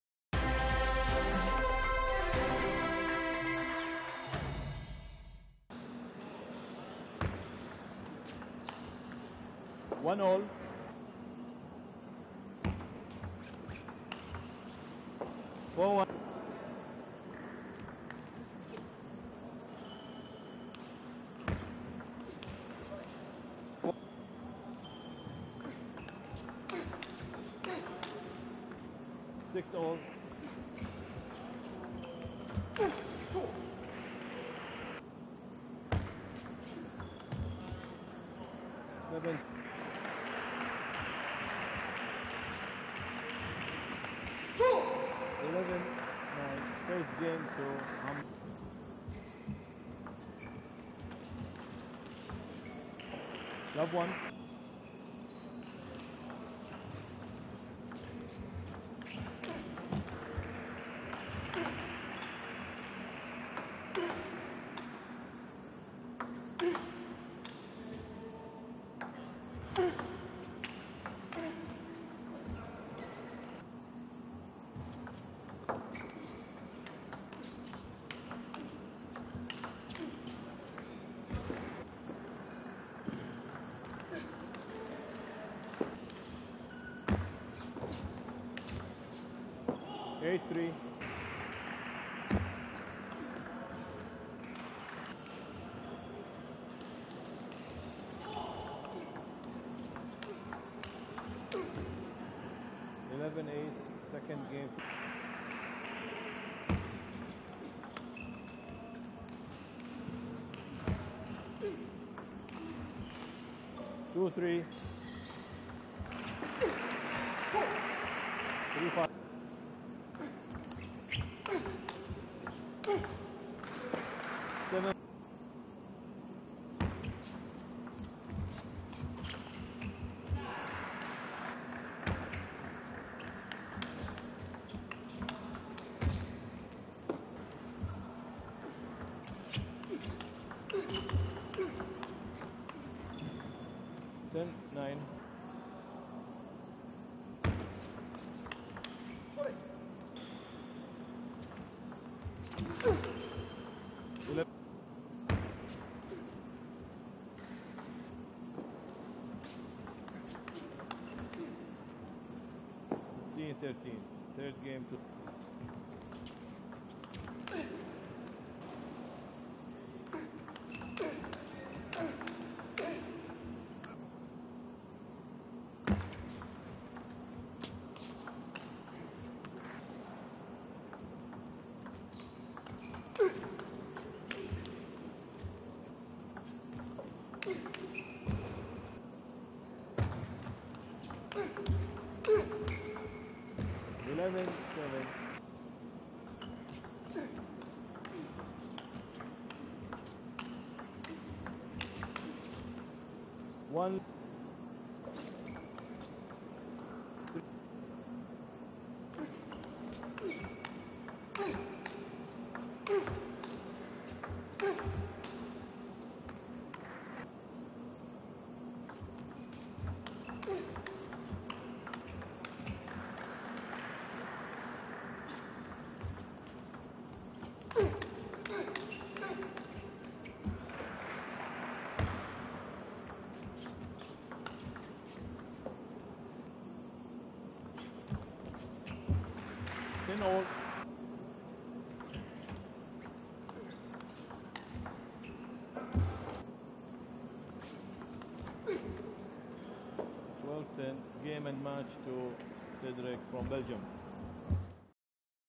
2016 Kuwait Open Highlights: Cedric Nuytinck vs Mohamed Shouman (Pre)
Review all the highlights from the Cedric Nuytinck vs Mohamed Shouman (Pre) from the Kuwait Open 2016